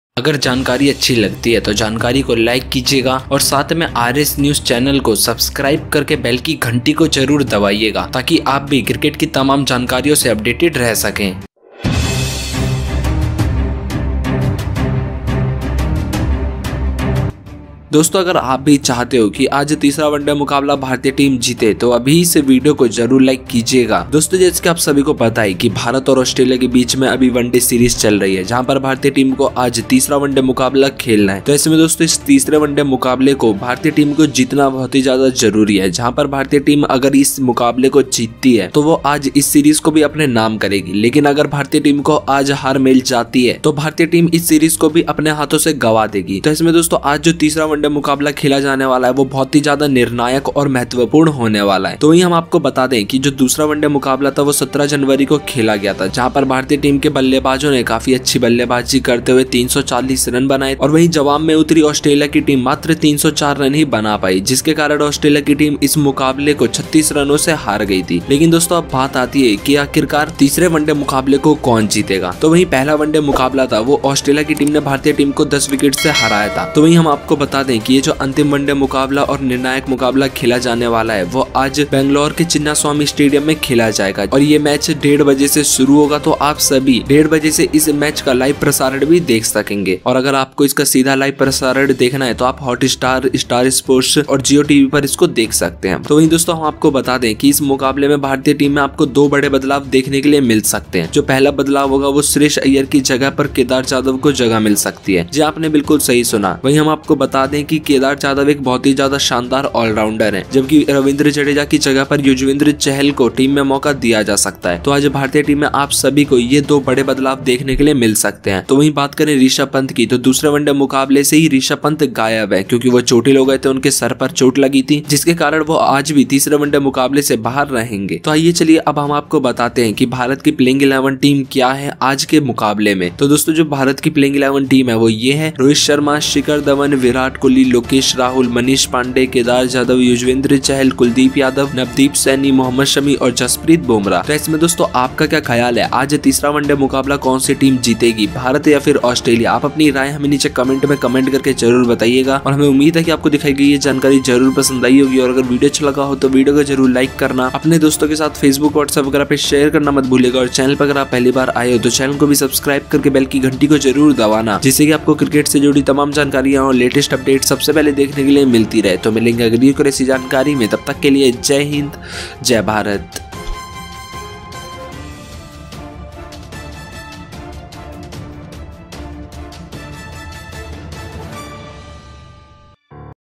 देखिये,ऑस्ट्रेलिया को मजा चखाने के लिए कोहली ने घोषित की तीसरे वनडे की प्लेयिंग 11 टीम,रोहित-धवन आए
ABOUT CRICKET NEWS:

The second ODI of the three-match ODI series being played between India and Australia was played in Rajkot. In this match, the Australian team was defeated by the Indian team by 36 runs and took the ODI series 1-1.

Now the final and decisive match of this ODI series is going to be played at Chinnaswamy Stadium in Bengaluru. This match will be played on January 19 from 1:30 pm Indian time.

Stormy batsman and wicketkeeper Rishabh Pant of the Indian team was injured in the first ODI. Due to which he is already out of the team.
It can be seen live on Hotstar Star Sport DD Sport and Jio TV sets. In this match, two changes can be seen in the Indian team. First, Kedar Jadhav can be replaced in place of Shreyas Iyer. Bat de Kedar Jadhav is a very brilliant all-rounder player. While Yuzvendra Chahal may get a chance in the team in place of Ravindra Jadeja.

Indian potential team: -

Rohit Sharma, Shikhar Dhawan, Virat Kohli (captain), Lokesh Rahul (wicketkeeper), Manish Pandey, Kedar Jadhav, Yuzvendra Chahal, Kuldeep Yadav, Navdeep Saini, Mohammed Shami and Jaspreet Bumrah.

Australia's potential XI:

Aaron Finch, David Warner, Marnus Labushane, Steve Smith, Alex Carey, Ashton Turner, Ashton Agar, Ken Richardson, Mitchell Starc, Pat Cummins and Adam Jumpa.